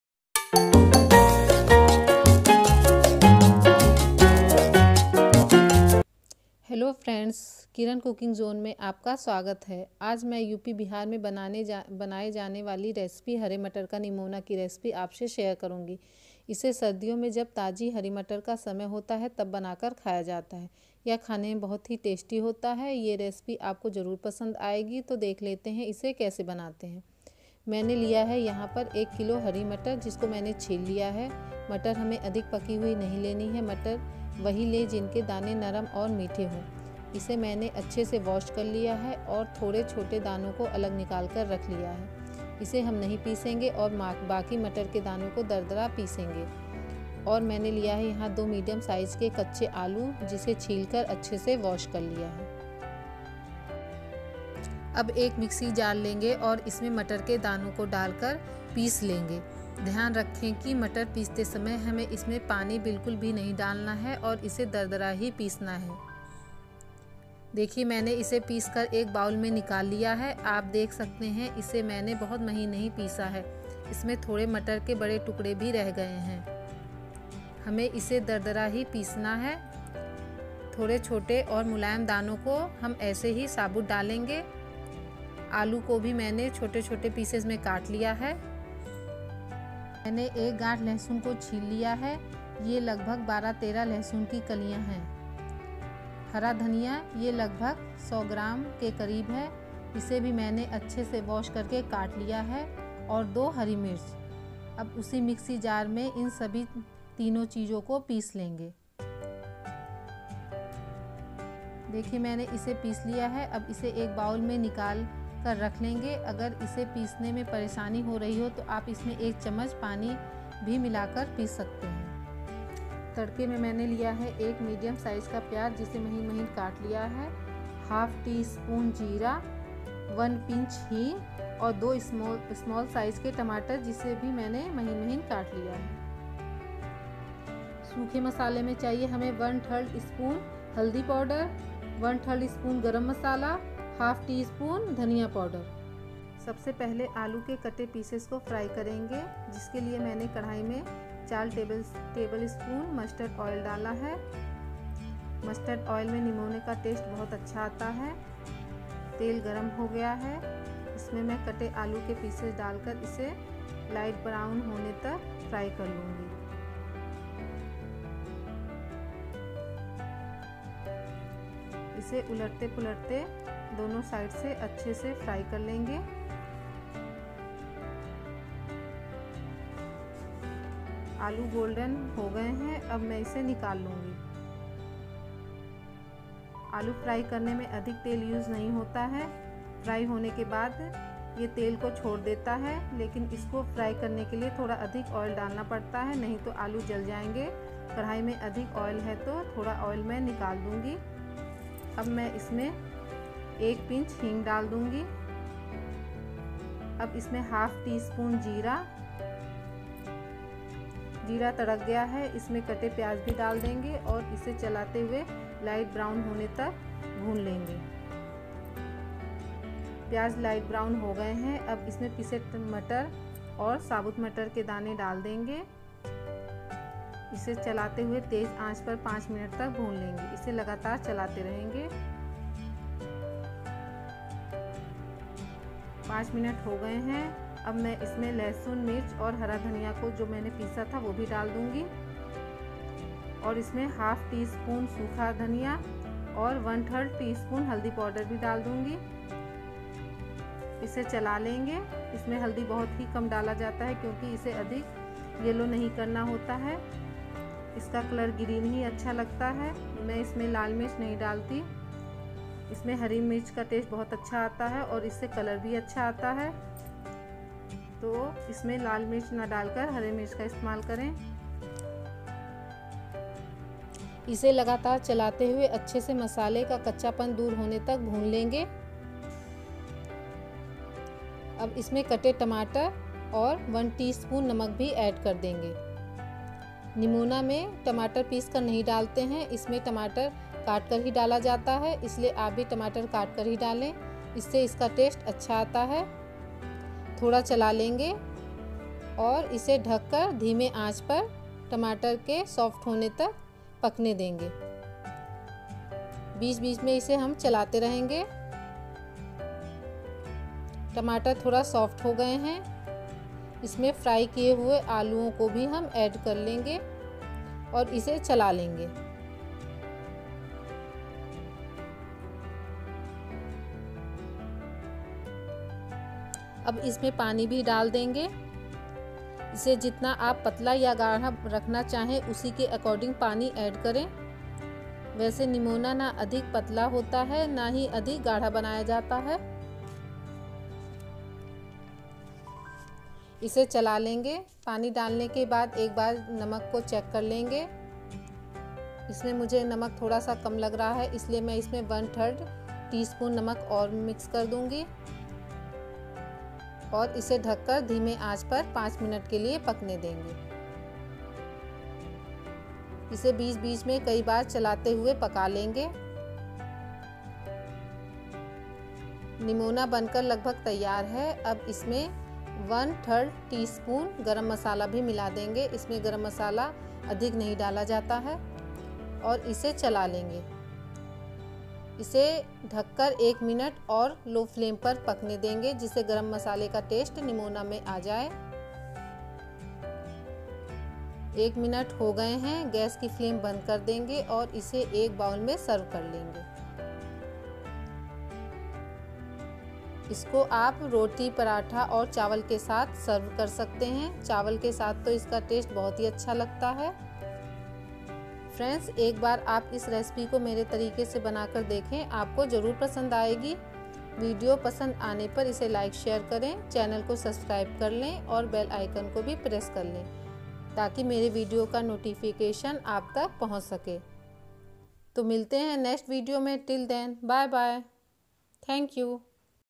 हरीमटर का निमोना बनाने का सबसे आसान और सही तरीका /Hari Matar ka Nimona.
Ingredients:---
1kg.Hari Matar
2 Tomato [Small Size]
2 Aloo [Small Size]
2 Green chilli
100 gm.Coriander leaves
15-16 Garlic
1Onion [Medium size]
1Pinch Hing
1/3 tsp Jeera
1/3 tsp Haldi powder
1/2 tsp Coriander powder
1/2 tsp Garam Masala
4 tbsp Mustard Oil
1-1/3 tsp Salt

Nemona Recipe in Hindi
How to make Nimona
Matar ka Nimona
Green peas Recipe
Hari Matar ka Nimona.